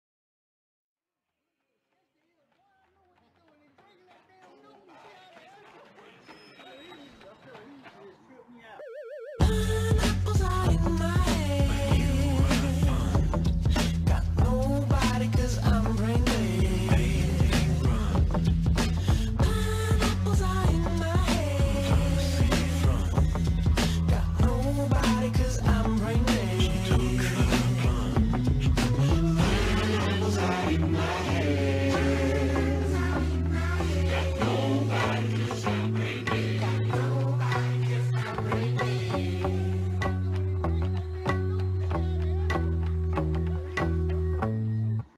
pork soda -.:MEME:.-
i just wanted to make something about my excitement and (slight) anger of the new gravity falls funko pops
im mostly happy tho